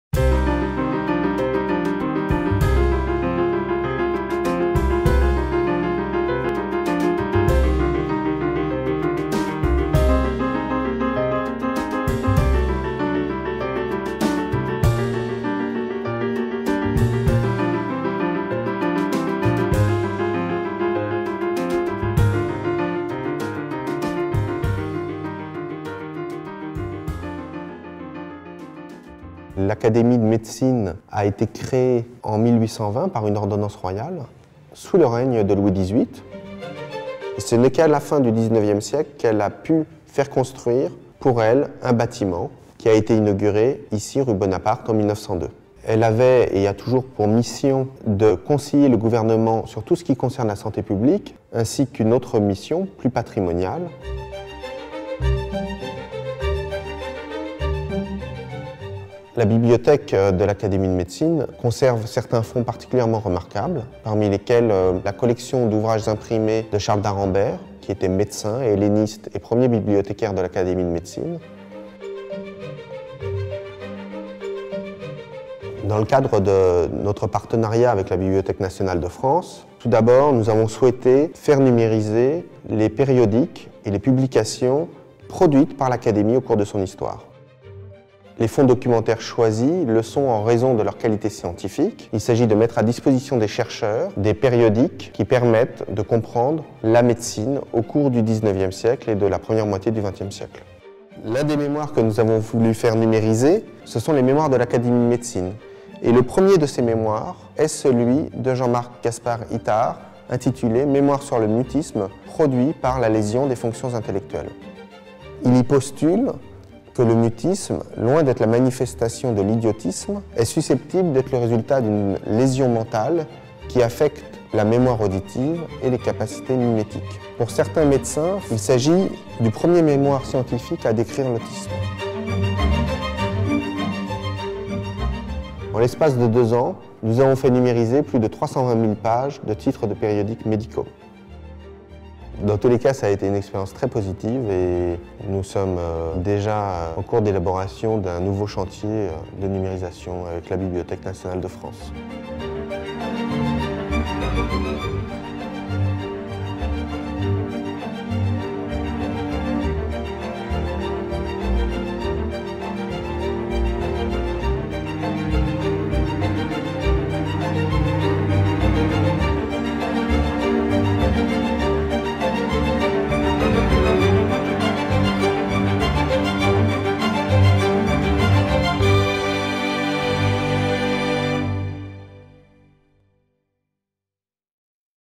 Les partenaires de Gallica - Académie nationale de médecine
La Bibliothèque de l'Académie de Médecine est nichée au cœur du 6ème arrondissement de Paris. Grâce à la coopération numérique initiée par la BnF et ce partenaire, les mémoires de l'Académie de Médecine sont disponibles en ligne sur Gallica. Jérôme Van Wijland, directeur de la Bibliothèque, présente les premiers travaux réalisés sur l'autisme.

Document présenté : Mémoires de l'Académie de médecine, "mémoire sur le mutisme produit par les lésions des fonctions intellectuelles" - 1828